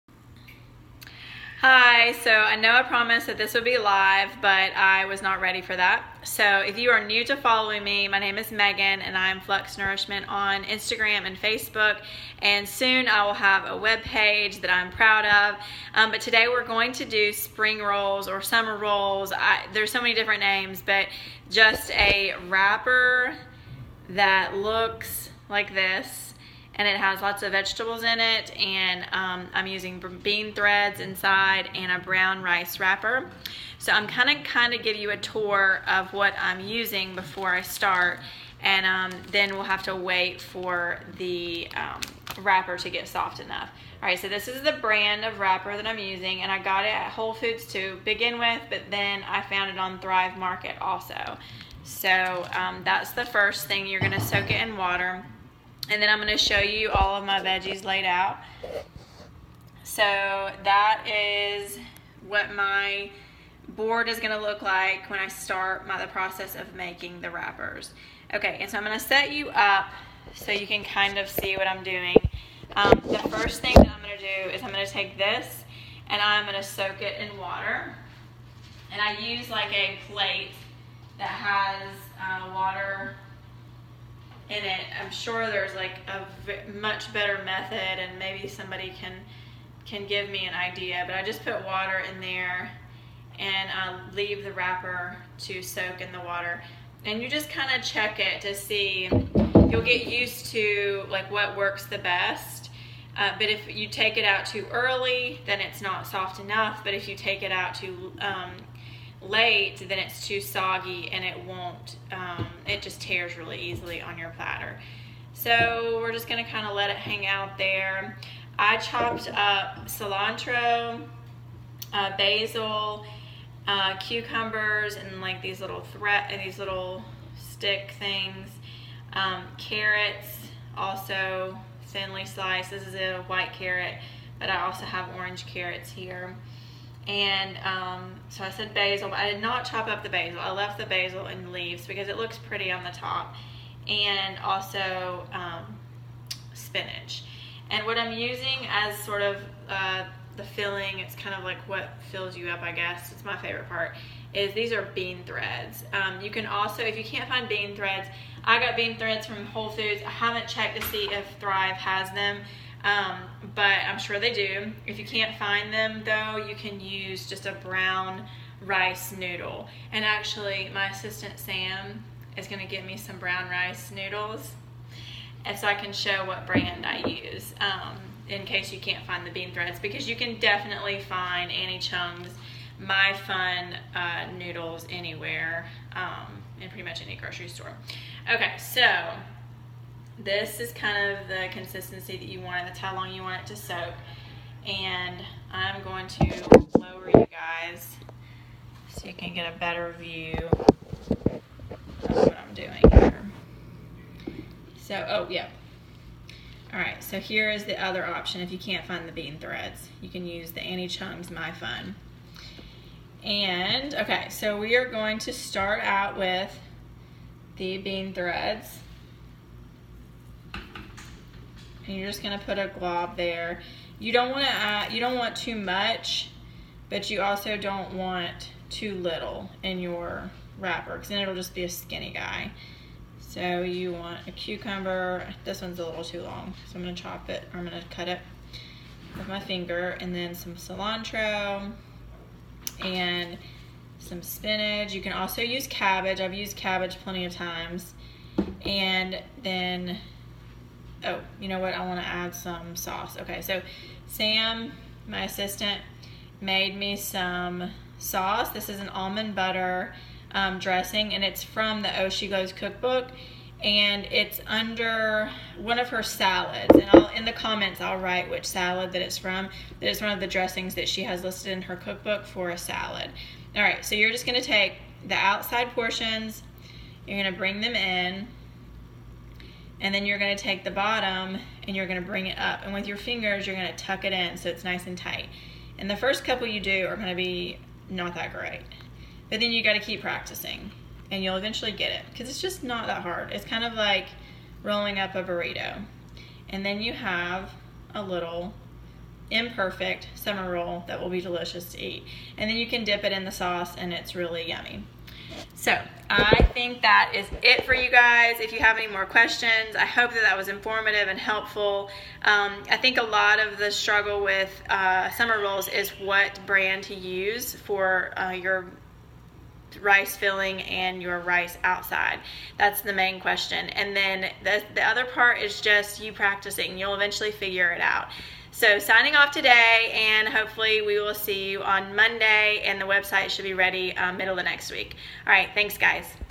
Summer Roll Demo
Megyn's tutorial on how easy it is to make summer rolls. They are healthy, tasty and easy to make!